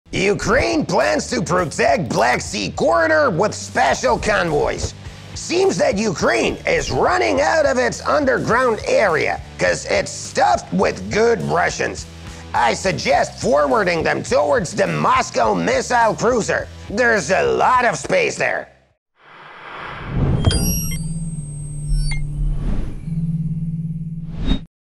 Ukraine's Bold Move! Special Convoys to Shield Black Sea Corridor | The Gaze
Hold on to your seats as Ukraine unveils a jaw-dropping plan to safeguard the Black Sea Corridor with special convoys!